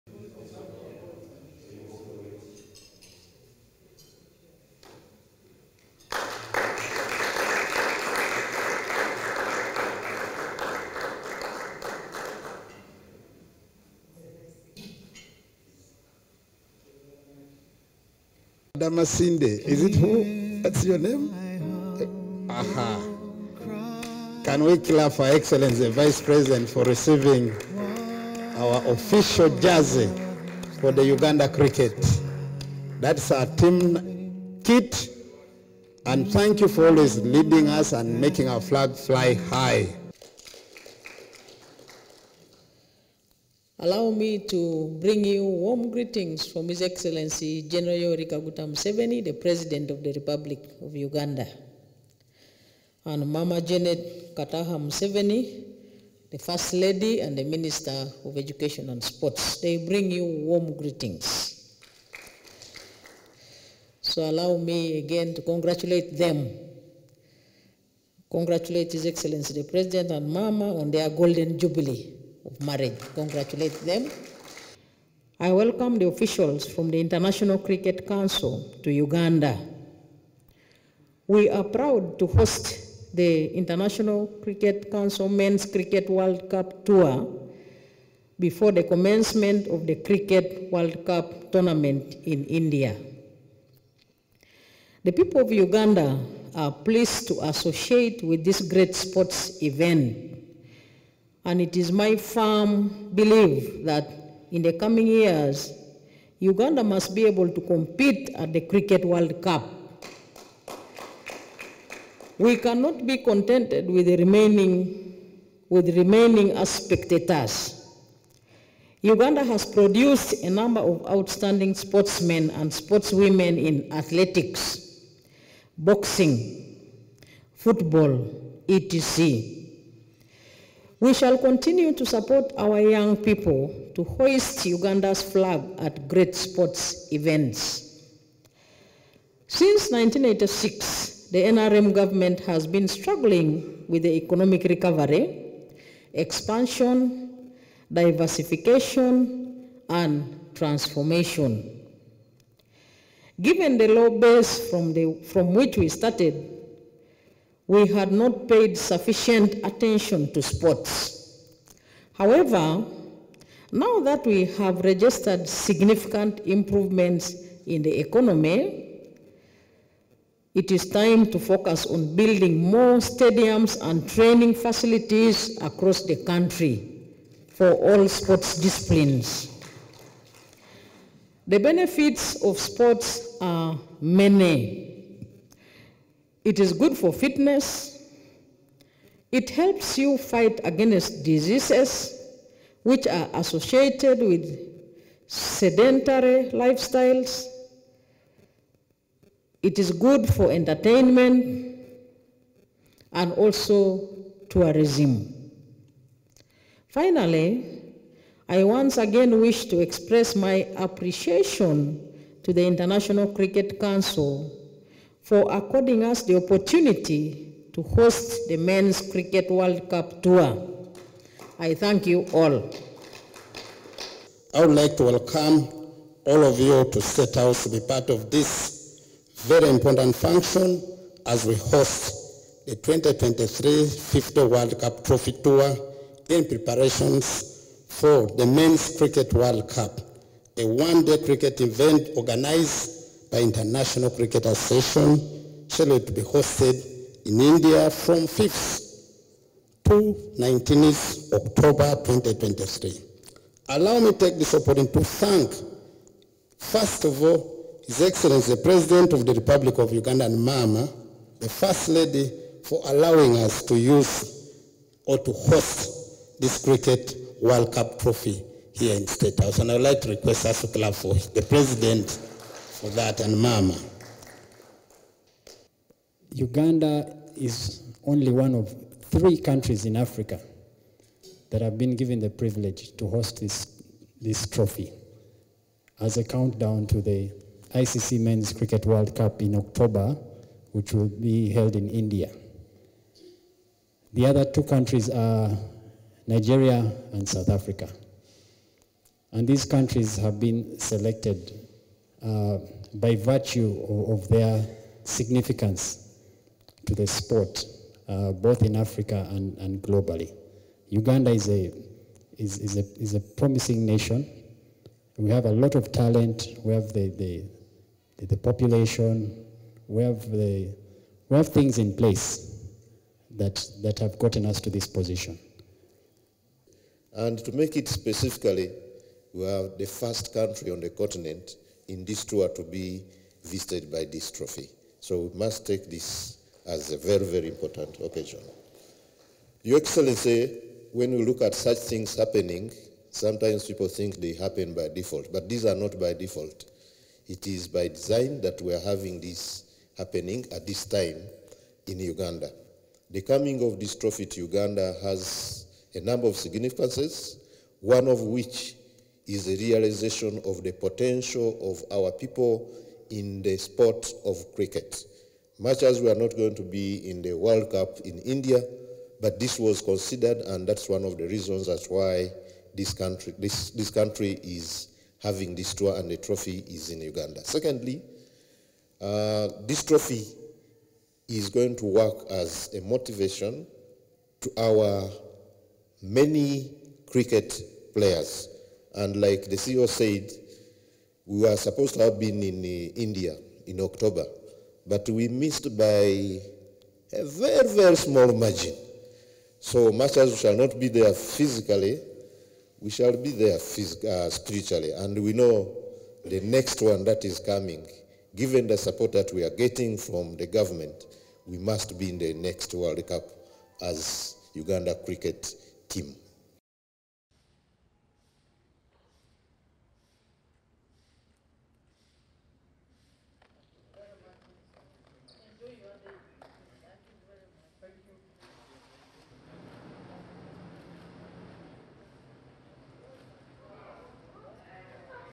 Cricket World Cup at State House Uganda: Museveni says Uganda must compete at the Cricket World Cup
President Yoweri Kaguta Museveni has today reiterated the
government's commitment to develop all sports disciplines in Uganda
now that the country has registered significant improvements in the
economy.

According to the President, the focus is now on building more stadiums
and training facilities across the country for all sports disciplines.
This was contained in his message delivered by the Vice President, Maj.
(Rtd). Jessica Alupo Rose Epel at State House Entebbe, at a function to
welcome the International Cricket Council (ICC) Men’s cricket world cup
trophy before the commencement of the cricket World Cup tournament
in India.
H.E Museveni expressed his appreciation to the international Cricket
council for giving Uganda the opportunity to host the men’s cricket world
cup tour. He said whereas the people of Uganda are pleased to
associate with this sports event (Cricket World Cup) Uganda should stop
being spectators.
“It is my firm belief that in the coming years, Uganda must be able to
compete at the Cricket World Cup. We cannot be content with remaining
as spectators. Uganda has produced a number of outstanding sports
men and women in different fields such as athletics, boxing, football etc.,
and we shall continue to support our young people to hoist Uganda’s flag
at great sports events,” H.E Museveni said.

President Museveni added that since 1986, the NRM government has
been struggling with the economic recovery, expansion, diversification
and transformation and therefore had not paid sufficient attention to
sports which have many benefits namely; fitness, fighting lifestyle
diseases, entertainment and tourism.
The Minister of State for Sports, Hon. Peter Ogwang said with the tour of
the prestigious Men’s World Cup Trophy, Ugandan youth and upcoming
players will be highly motivated to play cricket.
“As we host the World Cup trophy today, we are focusing on motivating
our people to fully embrace the spirit of sports and specifically the game
of cricket. Uganda is number one in Africa and we are the champions of
cricket as of now this year,” Minister Ogwang noted, adding that he’s
among those who will support a proposal to include Cricket as part of all
African games.
Minister Ogwang appreciated President Museveni for supporting sports
in Uganda especially recognizing athletes with monthly stipends of 5
million shillings for gold winners, 3 million shillings for silver holders and
one million shillings for bronze winners at All Africa Games, Olympics
and commonwealth games respectively.
The Assistant General Secretary in charge of Technical Affairs at
National Council of Sports, Mr. David Katende said the coming of the
Men’s Cricket World cup trophy to Uganda is so significant in terms of
realizing the country's potential in the sport of cricket much as Uganda
will not participate at the World Cup in India come October.
“This trophy is going to work as motivation to our many cricket players.
We were supposed to have been at the World Cup in India, but we
missed by a very small margin. Much as we shall not be there physically,

we shall be there spiritually. Given the support that we are getting from
the government, we must be in the next world cup,” Mr. Katende said.
The Chief Executive Officer of Uganda Cricket Association, Mr. Alan
Mugume paid tribute to his predecessors for the work leading up to the
level of cricket sport in the country to the extent of getting international
recognition.
“Uganda is a promising nation. We have a lot of talent and everything in
place that has gotten us to this position,” Mugume said.
Uganda is the first country on the continent out of the only three
countries in Africa and 20 countries around the world that have been
given the privilege to host the Men’s Cricket World Cup Trophy as a
countdown to the ICC world cup in October to be held in India. The other
countries on the continent are Nigeria and South Africa, selected by
virtue of their significance to the sport both in Africa and globally.
The Cricket World Cup Trophy will be in Uganda up to August 29th,
2023, and will be in the cities of Kampala and Jinja and some iconic
tourism sites with a sole purpose of identifying with the local cricket
fraternity and inspiring the next generation of cricketers.
The 2023 Cricket World Cup will be hosted by India from October 5 to
November 19.
The ceremony was also attended by officials from the National Council
of Sports, Uganda Cricket Association, the International Cricket council
and the Ministry of Education and Sports.